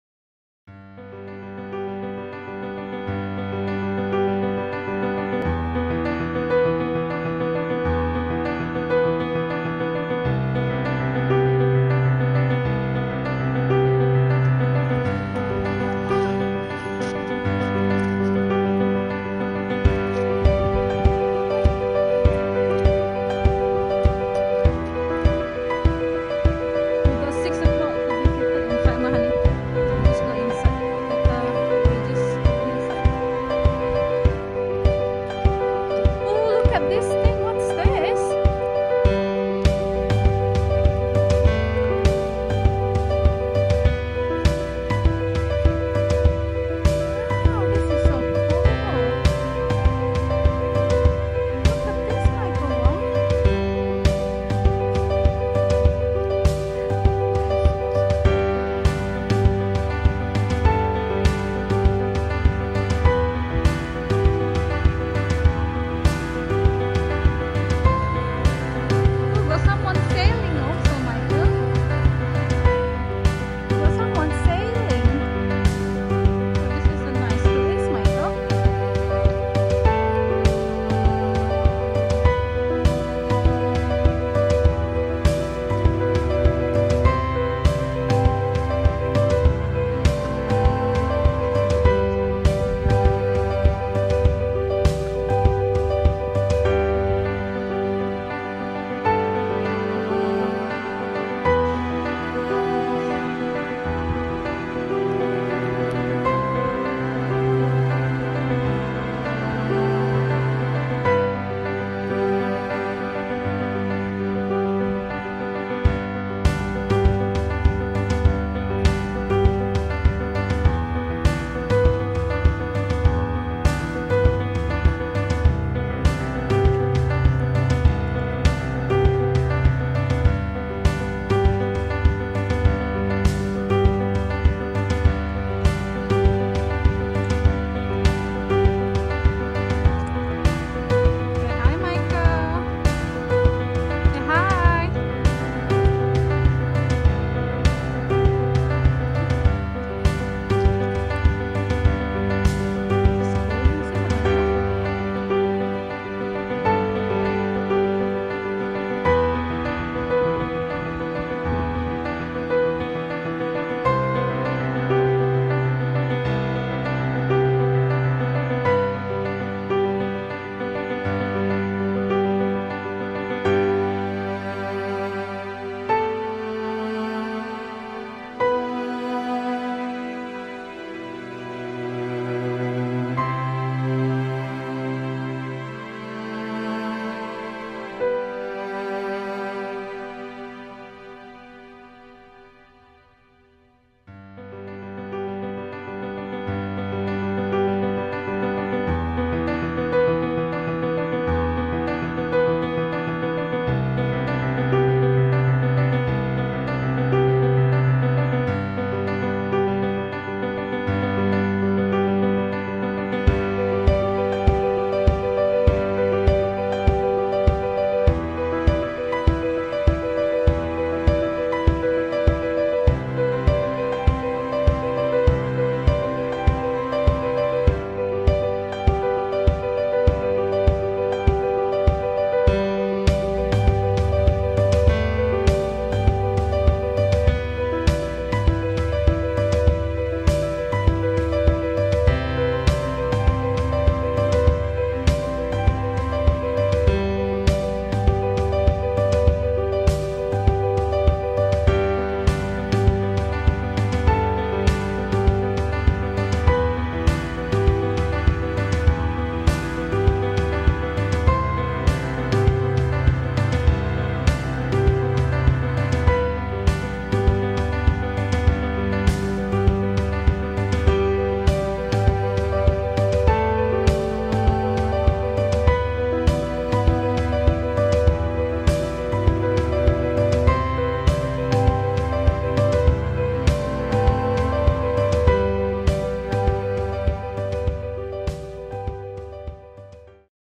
LA MARINA, Y FELINHELI | Port Dinorwic Marina | What we eat in WALES??!!
We found La Marina on our third day in Wales. It's located just a few minutes from
La Marina is a tapas and seafood restaurant.

Stay tune to see our vlog in Wales!

Michael & Mommy